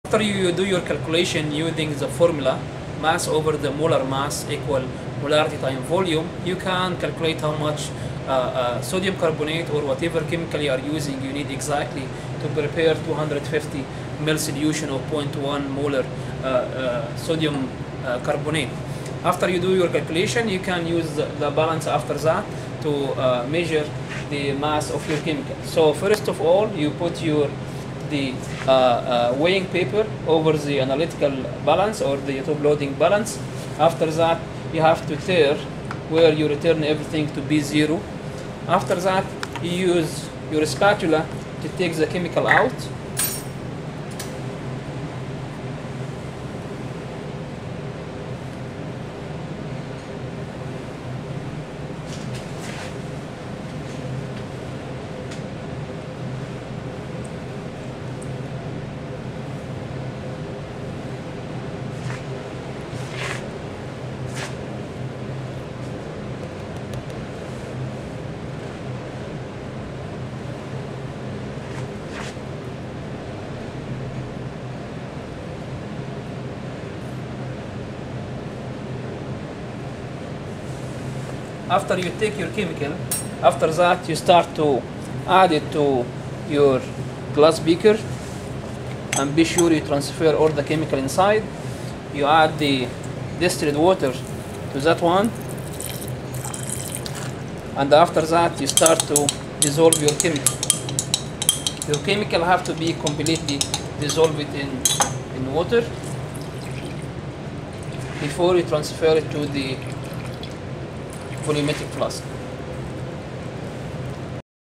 KAU CHEM 281 Mass & Volume Experiment part 2.MOD
In this part you will prepare weigh and dissolve the amount of Na2CO3 needed to prepare 0.10 M 250 ml solution.